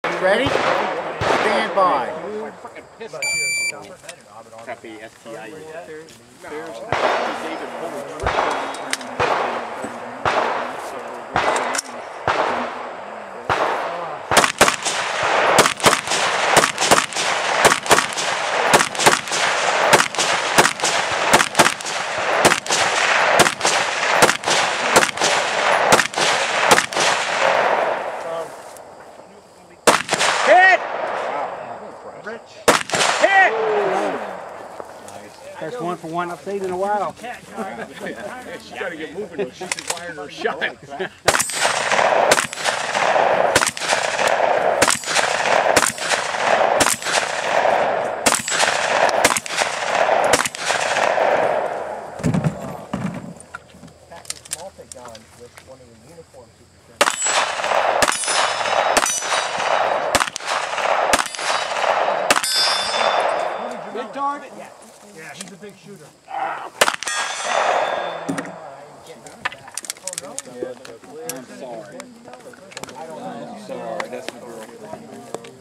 Gisela Stage 9 FNH 2013 Silent Hill
FNH 2013 Championship, Glengary WV, Silent Hill Stage 9

Raw 63.62
Penalties 0